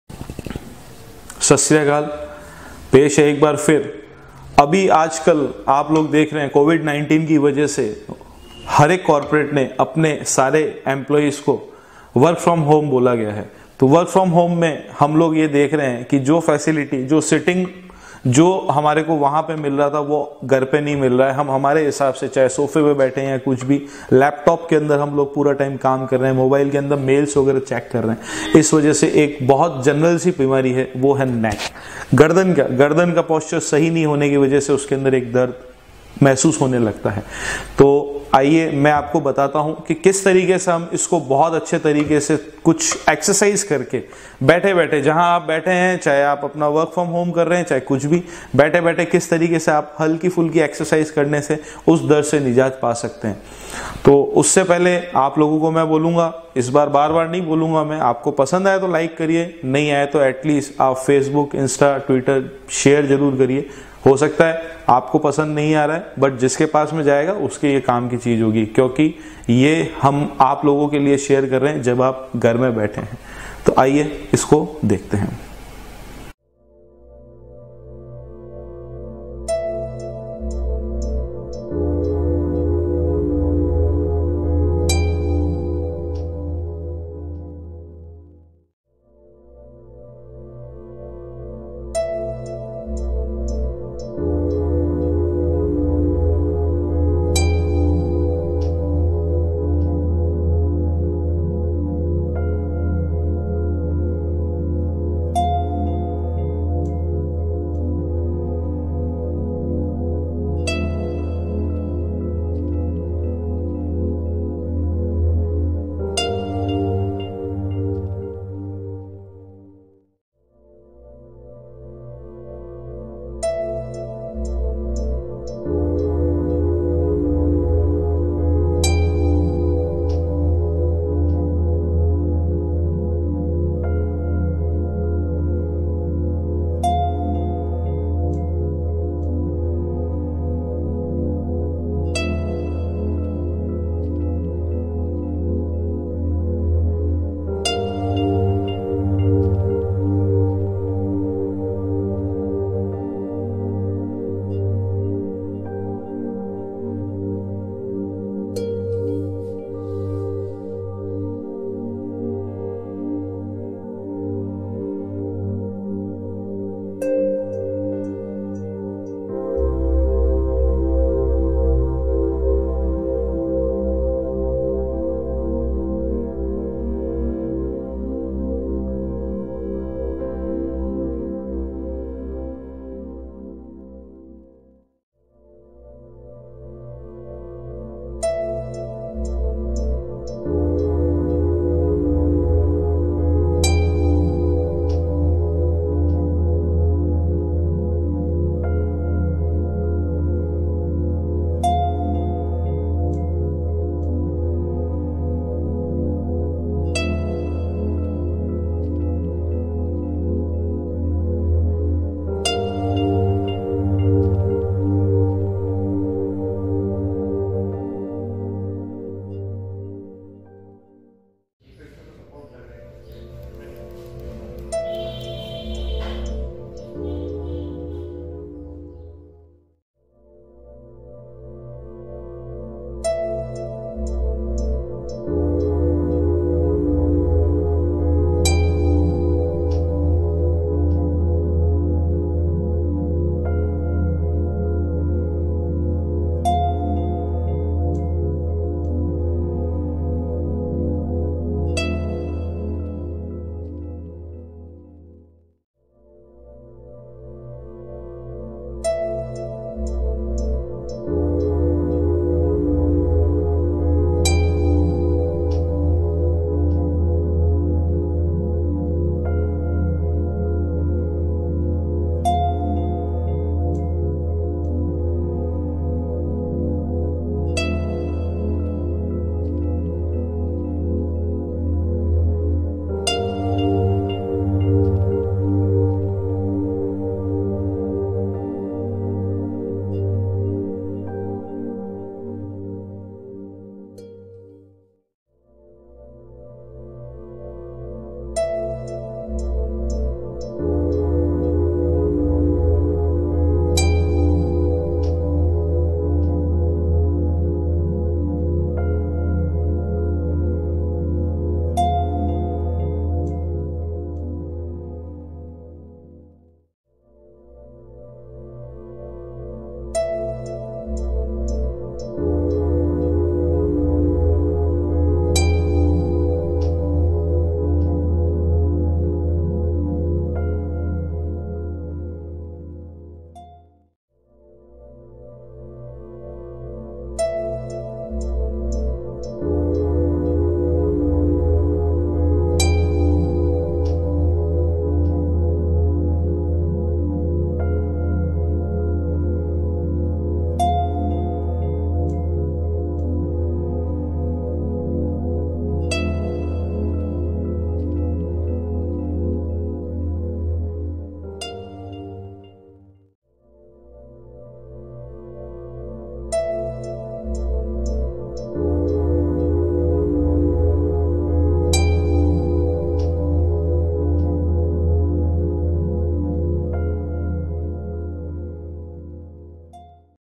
Neck pain exercises /home management
Simple exercises to do at your place these are good for corporates, bank workers, students, teachers and for house wives also to get free from your neck pain. Easy to understand and very easy to apply.